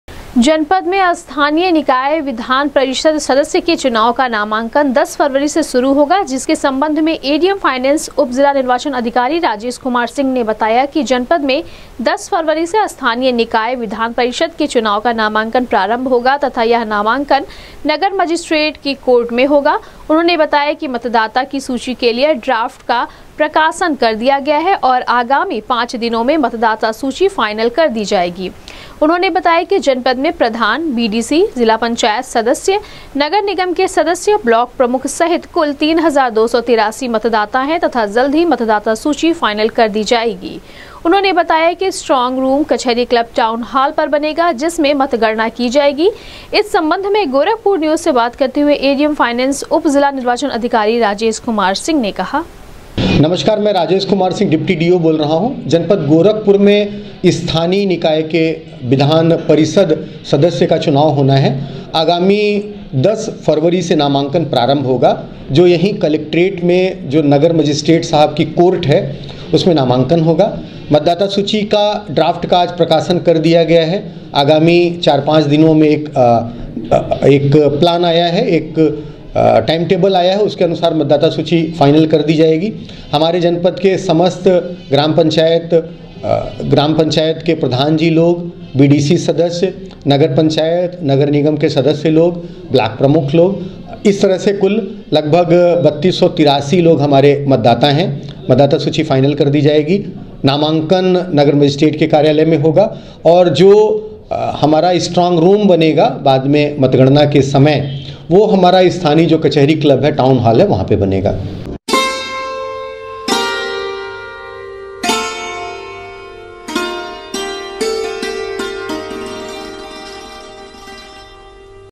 दस फरवरी से स्थानीय निकाय विधान परिषद सदस्य के चुनाव के लिए कलेक्ट्रेट परिसर के नगर मजिस्ट्रेट के
दस फरवरी से स्थानीय निकाय विधान परिषद सदस्य के चुनाव के लिए कलेक्ट्रेट परिसर के नगर मजिस्ट्रेट के कोर्ट में होगा नामांकन,एडीएम फाइनेंस/उप जिला निर्वाचन अधिकारी ने दी जानकारी